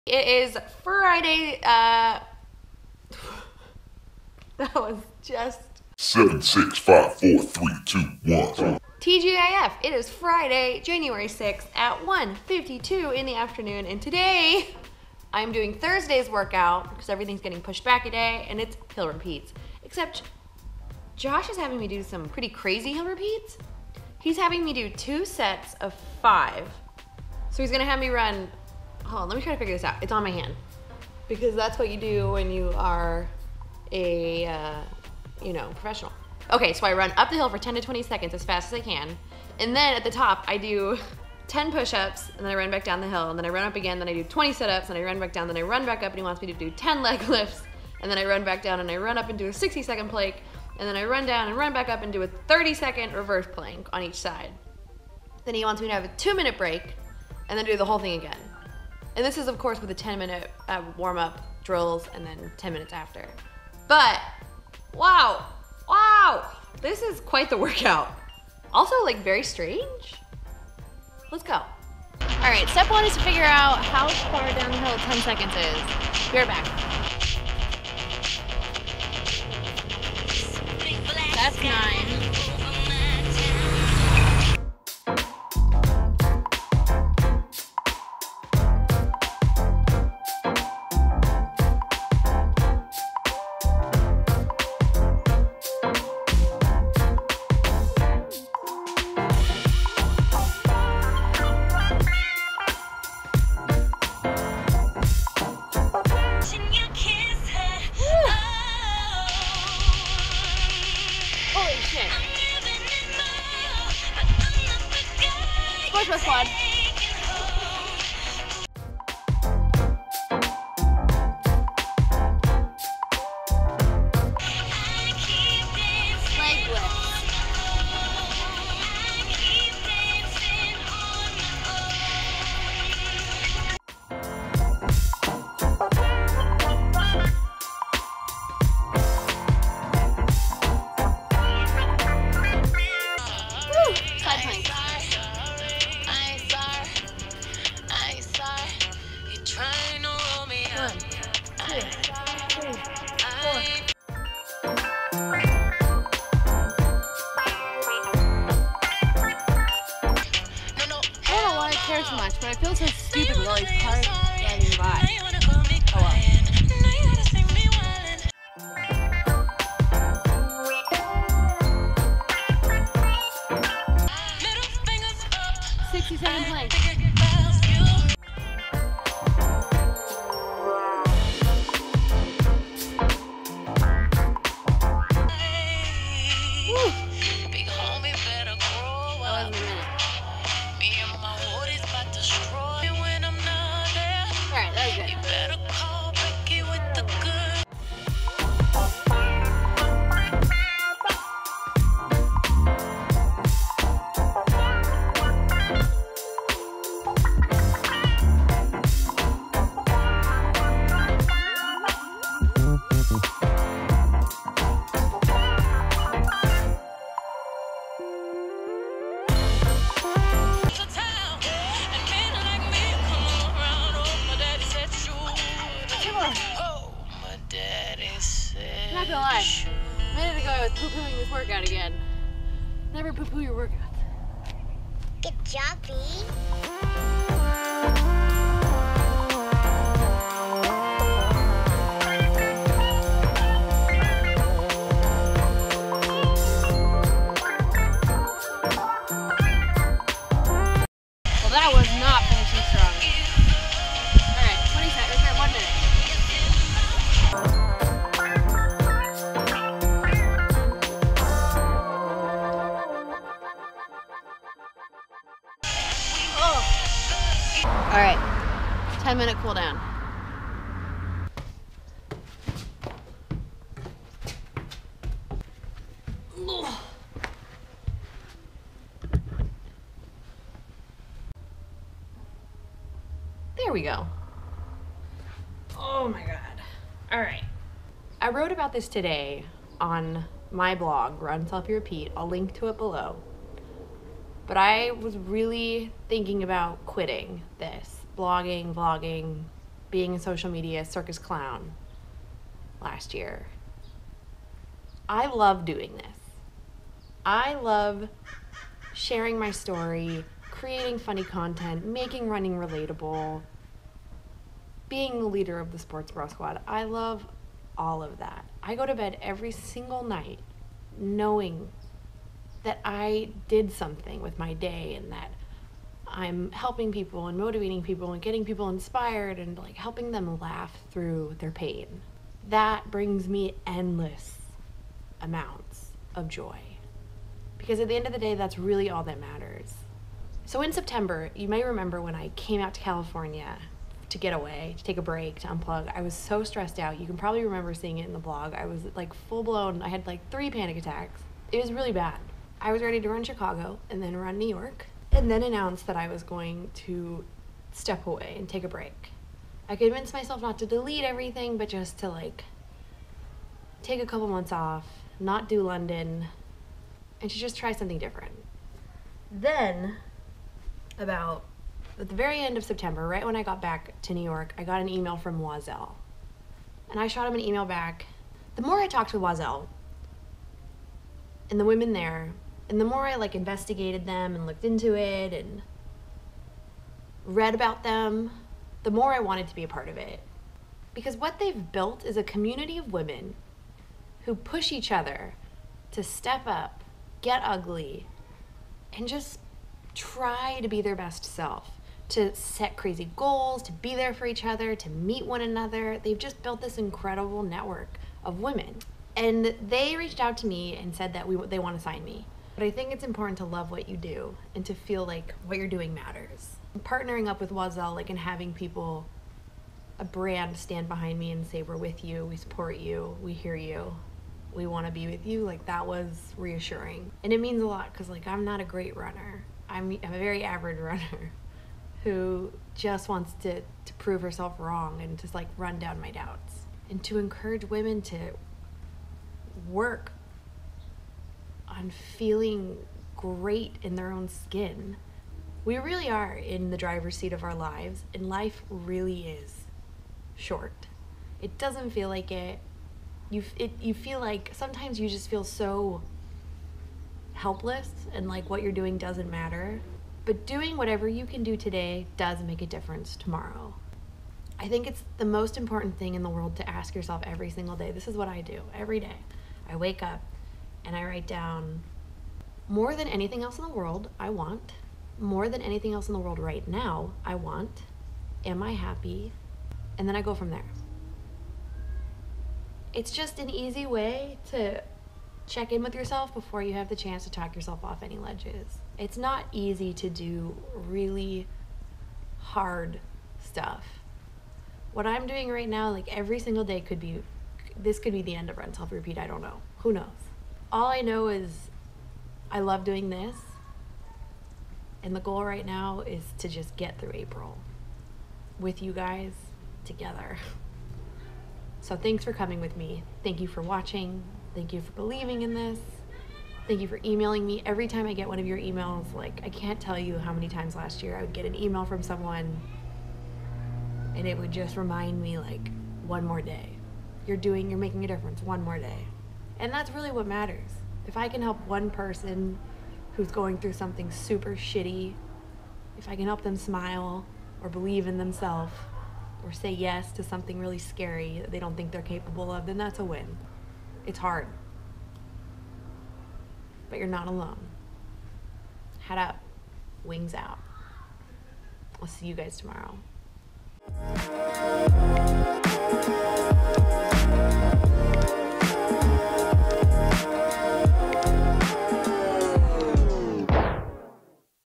Short Hill Repeats BQ OR Bust 142
2 x 5 sets of 10-20 second hill repeats with push ups, sit ups, leg lifts, side planks, and planks between each repeat.

I thought it would be easy. I was wrong.